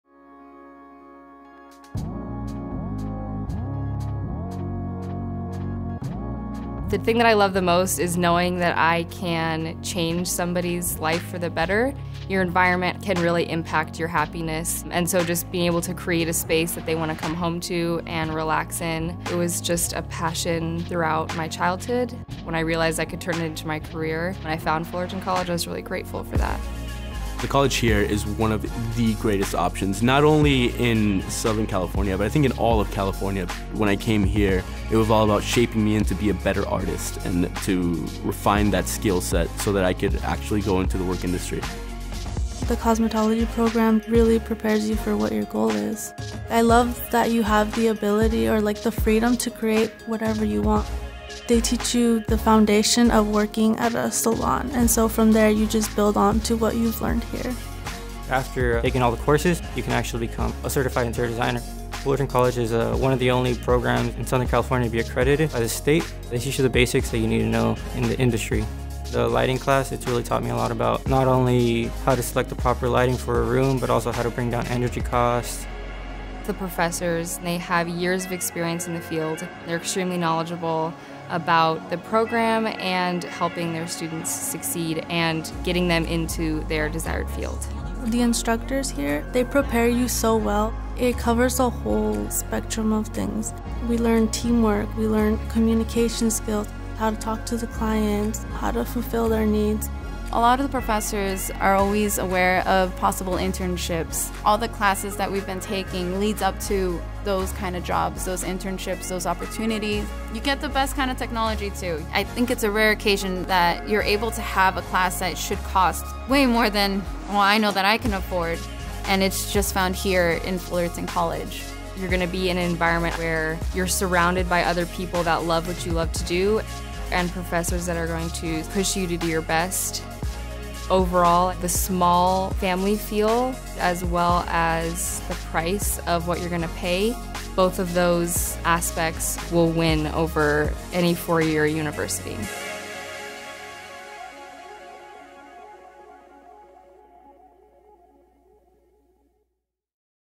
Launch Your Career at Fullerton College
Fullerton College is a proven regional leader in career technical education. Among its 140 career education programs, Fullerton College enrolled the most career education students in Orange County in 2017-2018. Career education programs are growing and thriving at Fullerton College thanks to bolstered financial support from the state and regional consortium. From the development of an internship program, to the creation of new industry-ready programs and the launch of a full-scale marketing campaign – Fullerton College is positioned to prepare students for the evolving needs of local industries and beyond.